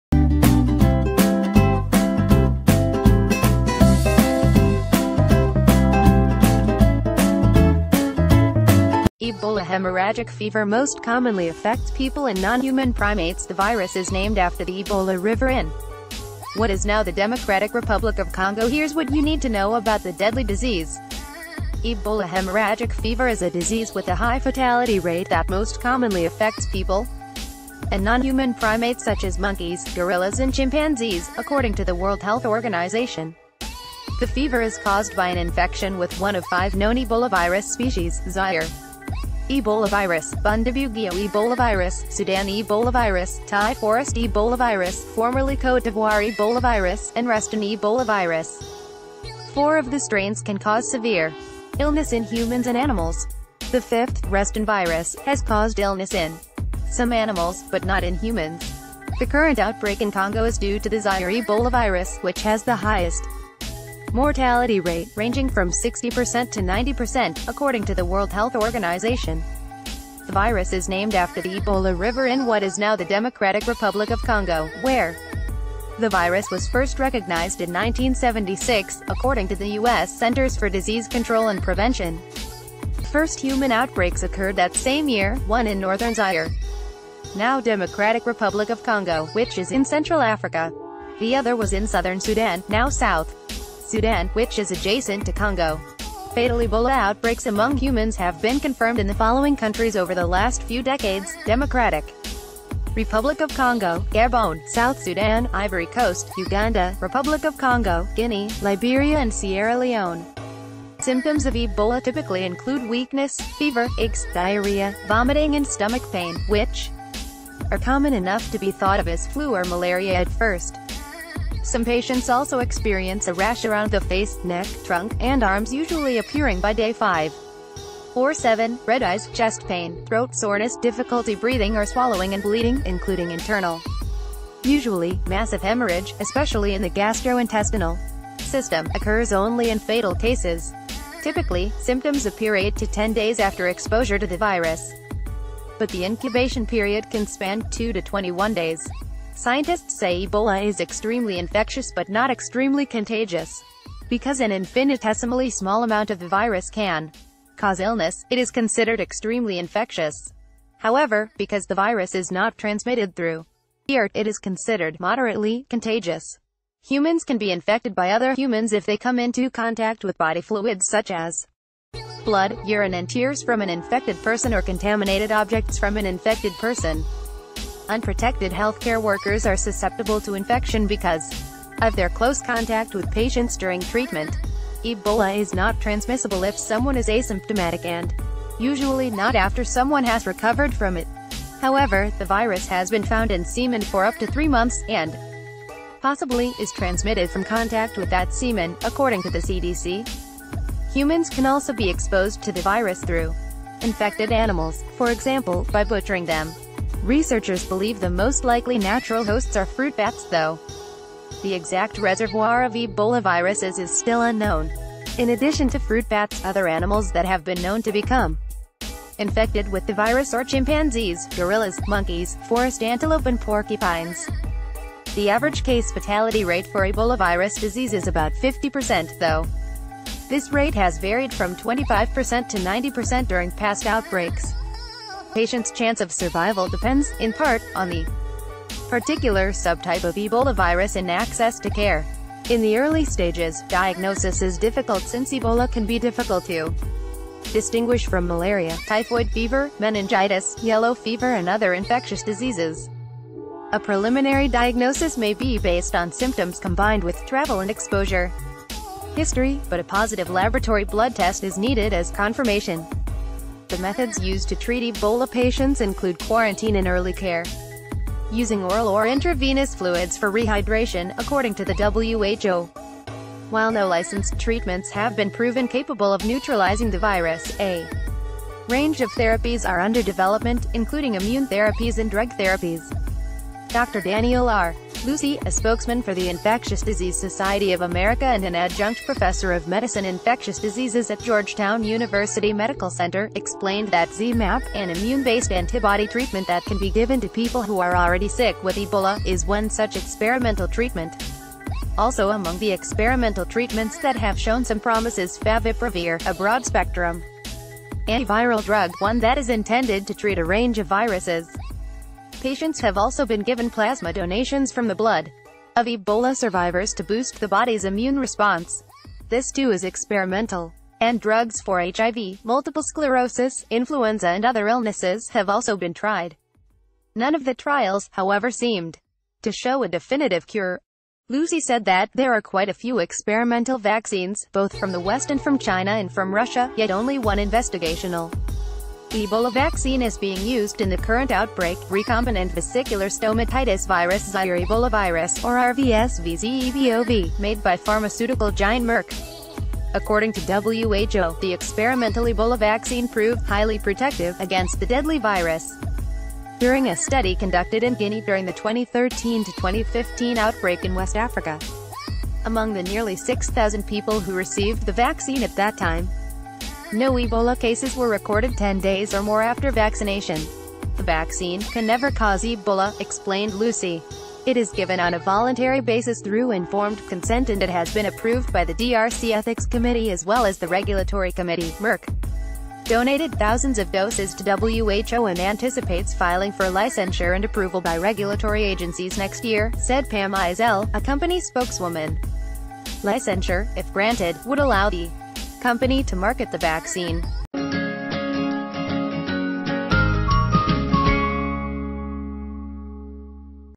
Your Ebola outbreak questions answered here
► Ebola hemorrhagic fever most commonly affects people and nonhuman primates The virus is named after ...